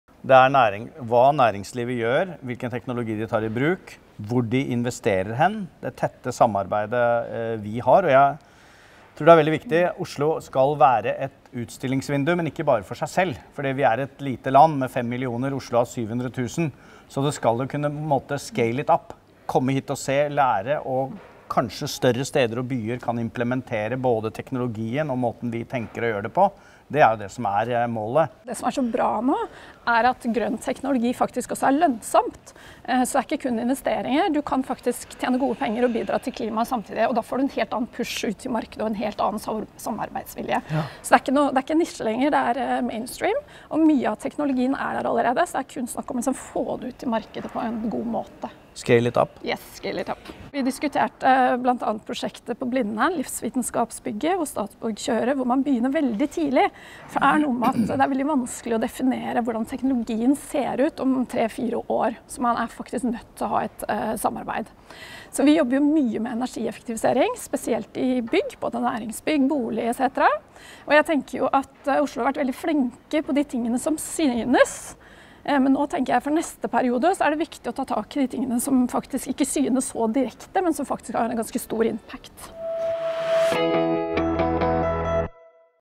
Karoline og Raymond Johansen om samspill og grønt lederskap | Schneider Electric
At vi samspiller om grønt lederskap er avgjørende. Se første del av praten til Karoline og Raymond Johansen, og les mer om vår IoT-baserte arkitektur og plattform EcoStrucure for bygg, datasentre, infrastruktur og industrier her